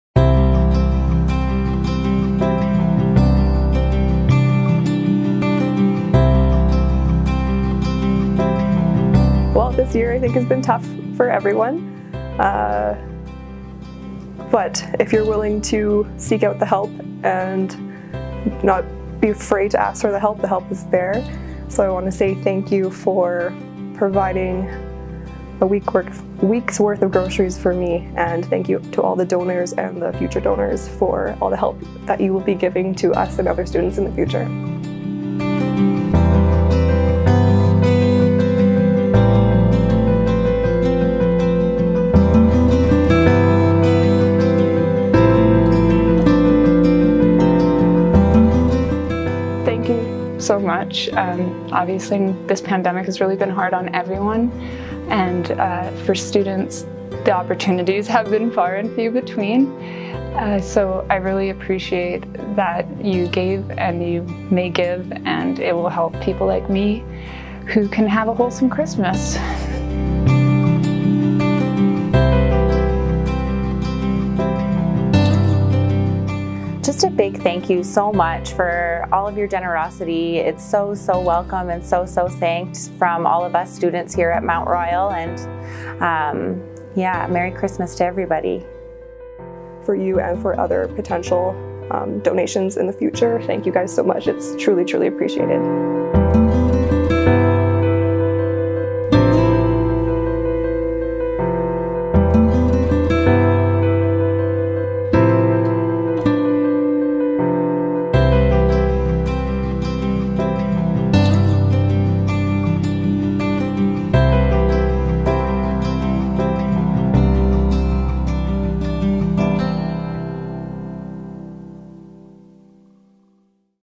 Fundrace Flurry!
SAMRU’s seasonal fundraiser to support MRU students during a financially stressful time of year-- the holidays! Get ready to race your fellow MRU campus community members as your team attempts to raise the most donations for the SAMRU Peer Support Centre. The Peer Support Centre provides many critical services for Mount Royal students including, but not limited to, food and financial security programs.